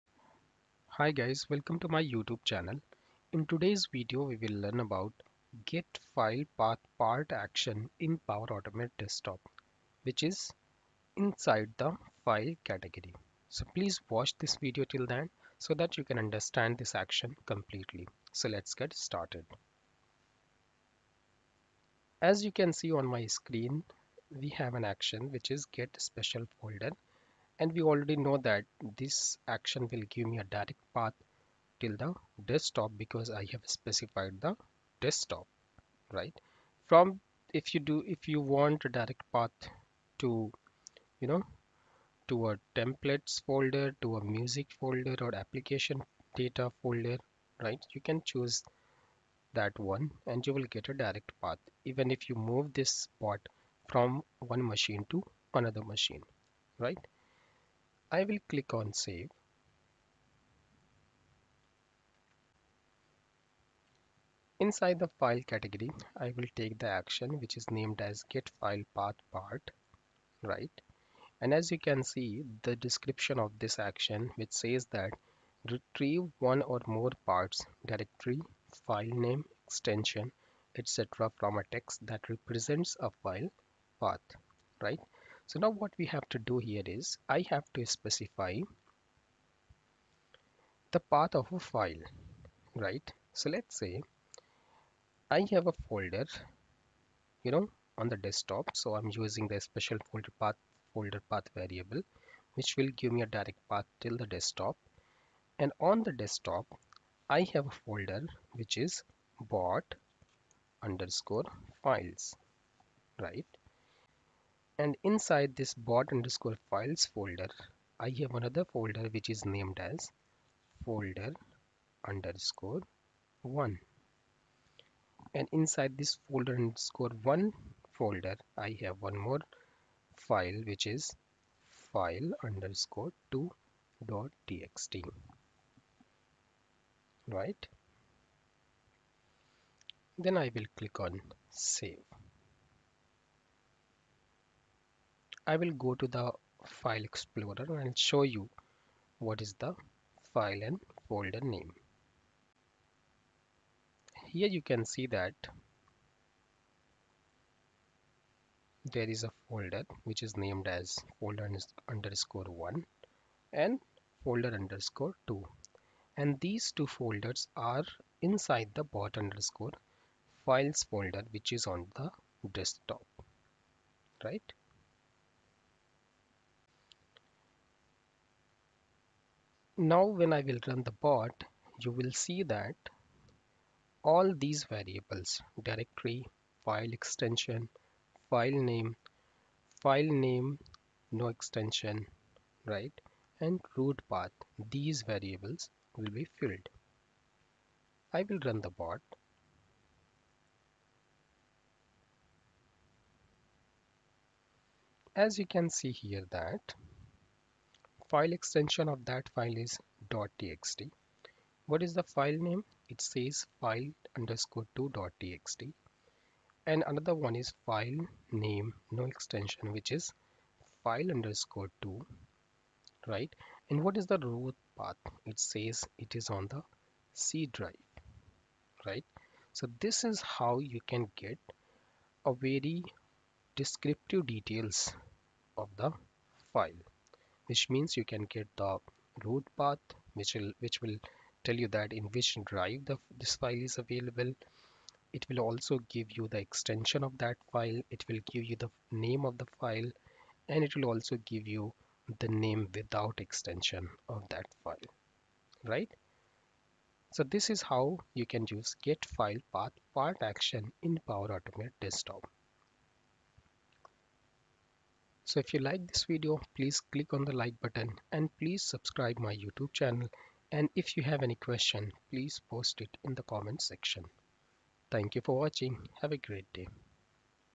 Get File Path Part Action In Power Automate Desktop
In this Video, you will learn about Get File Path Part action which gives File name, File extension, Root Path and directory. Please watch this video till the end to understand it complete.